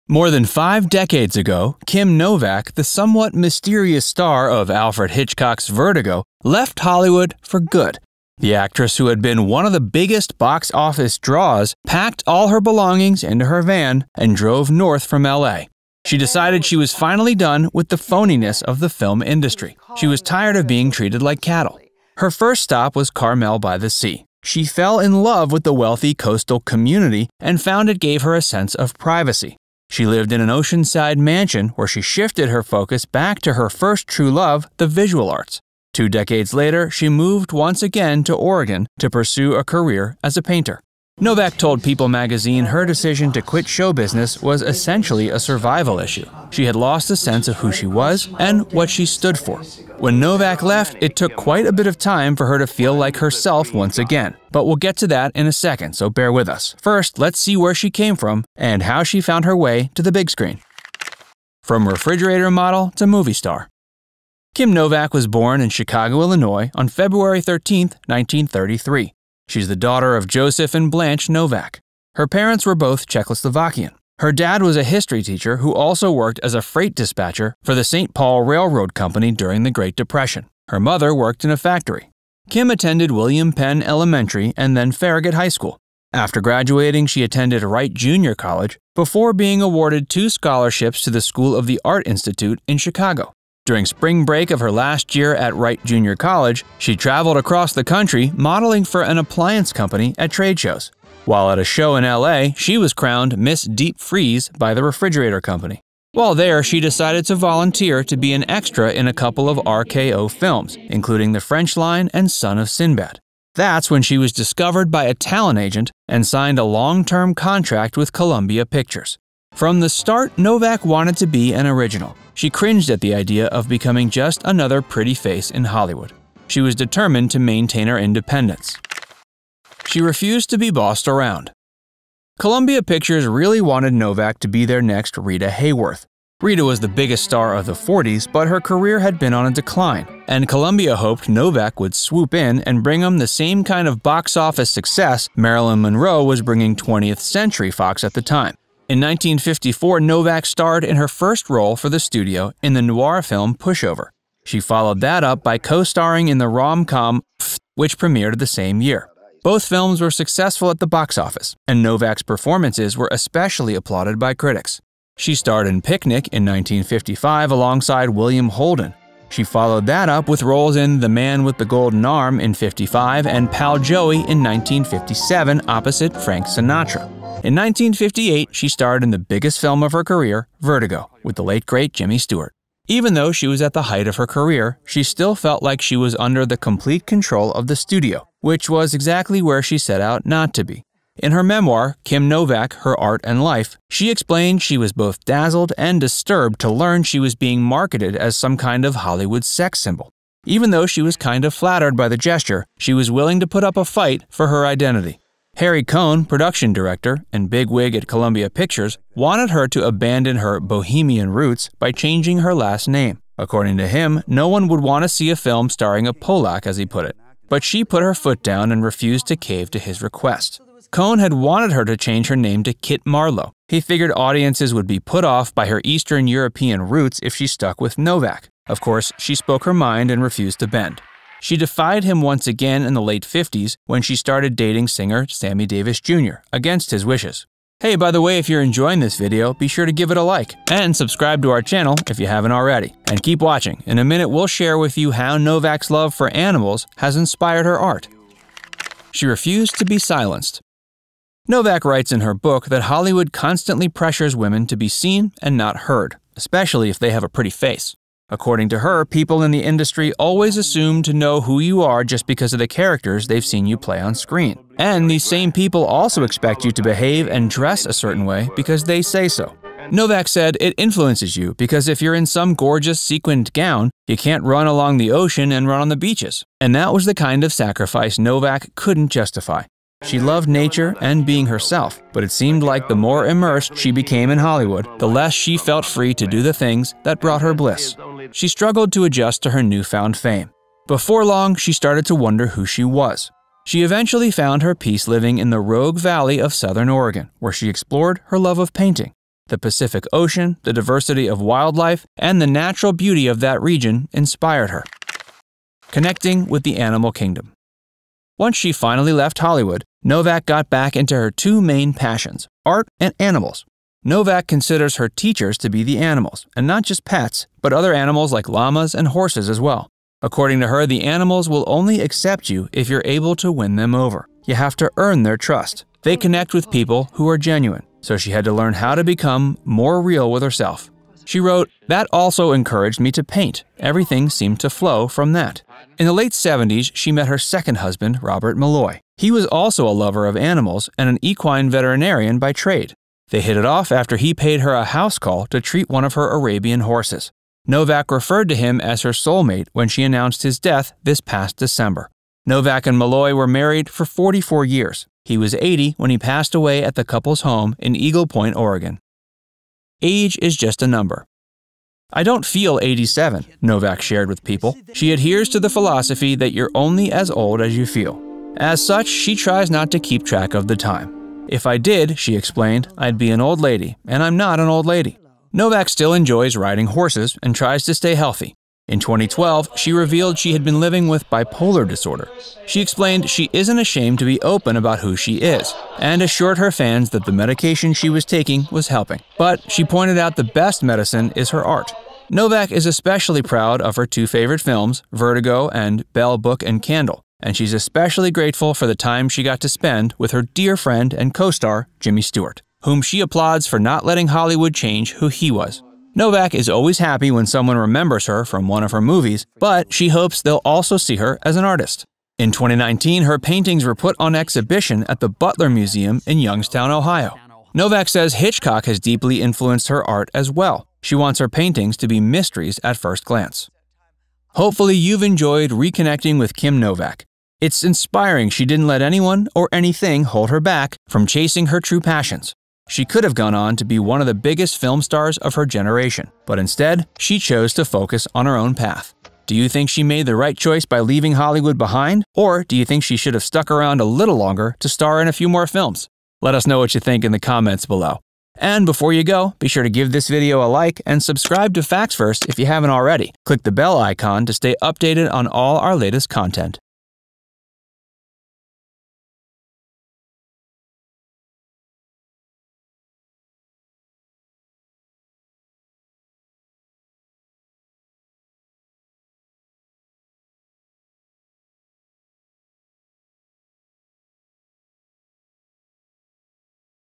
Why Kim Novak Quit Hollywood
A little more than 50 years ago, Kim Novak, star of Alfred Hitchcock's Vertigo left Hollywood for good. She decided she was done and that meant she was done. Even though she had once been the number one draw at the box office, she packed all of her belongings into her van and drove north out of LA on the 1.

She found her way to Carmel-By-The-Sea, where she fell in love with the wealthy coastal community and found that it gave her a sense of privacy that she had been missing. She lived in an ocean-side mansion where she shifted her focus back to her first true love, the visual arts. Two decades later, she moved once again to Oregon to pursue a career as a painter.

Hollywood had stripped her of her identity. By the time she left, she had to pick up all of the pieces of who she was and rebuild from the ground up. It wasn't a simple process either. Hollywood constantly tries to change you. When Novak left Hollywood for good, it took quite a bit of time for her to feel like herself once again, but eventually, she was able to find herself once again.